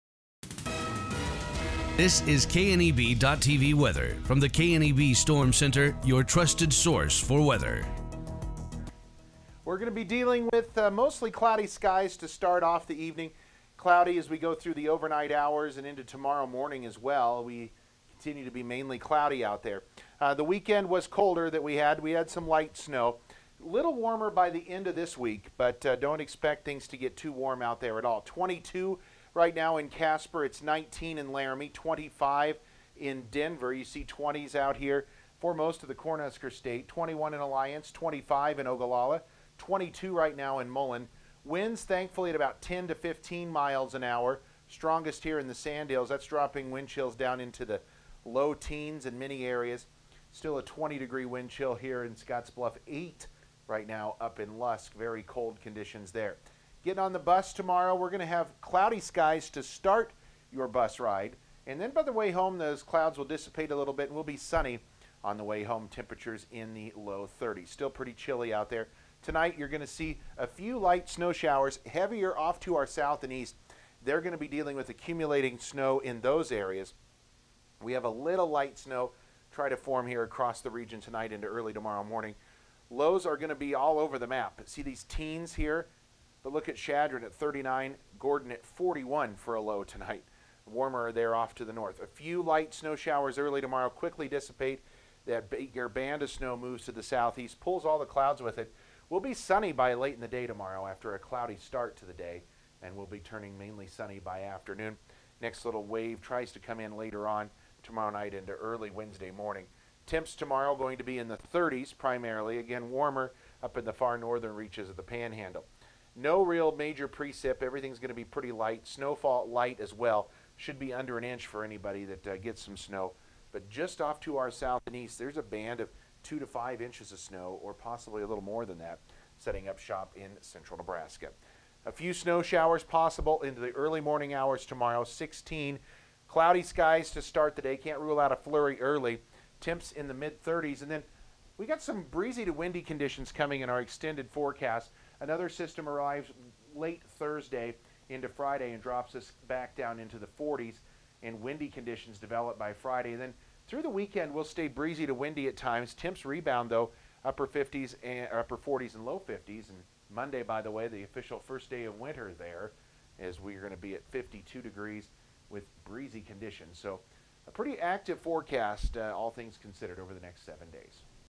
Monday Evening Forecast- December 14, 2020
Bill Boyers says it will be a very active weather week. He breaks it all down right now on KNEB.tv Weather: